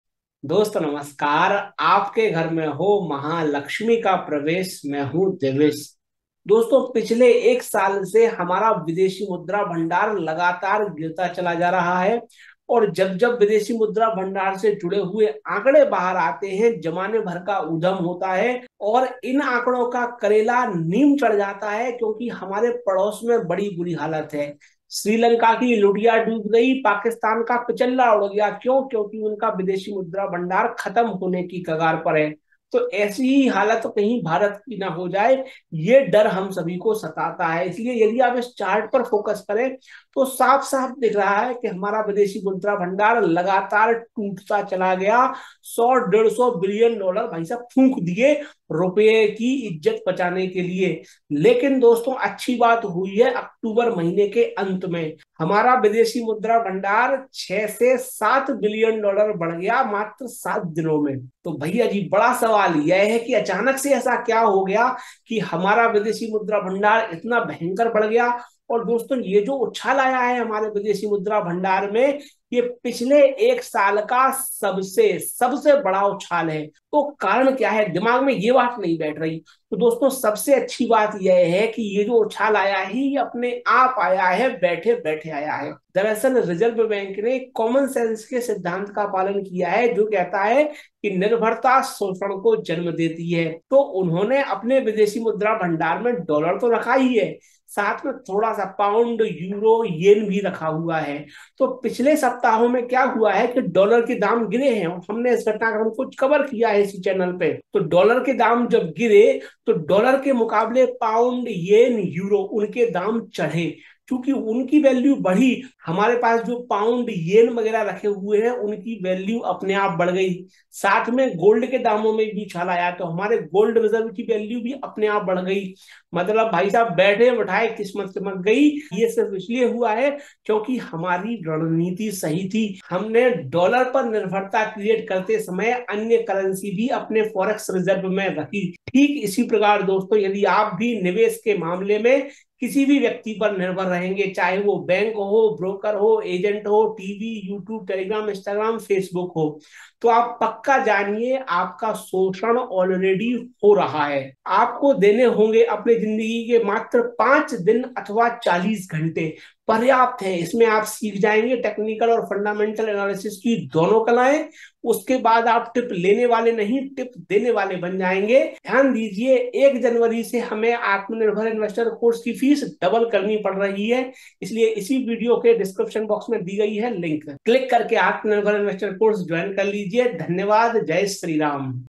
बैठे-बैठे कुबेर का खज़ाना खींचा | Analysis By Devesh
Topic - Indian forex reserves post the highest jump in last 1 year- Will it help aam aadmi?

In this Devesh has discussed and analyzed